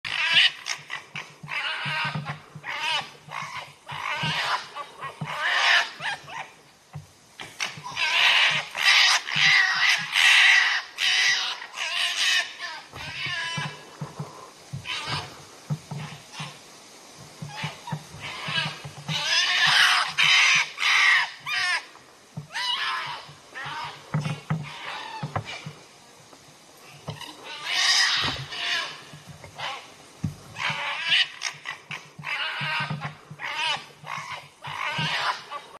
Furious Fox Fight Screaming in Jungle
Some foxes are strictly territorial and will attack another individual as soon as they step across the boundary into their range. Others only fight when an intruder breaches their core area or is spotted stealing food.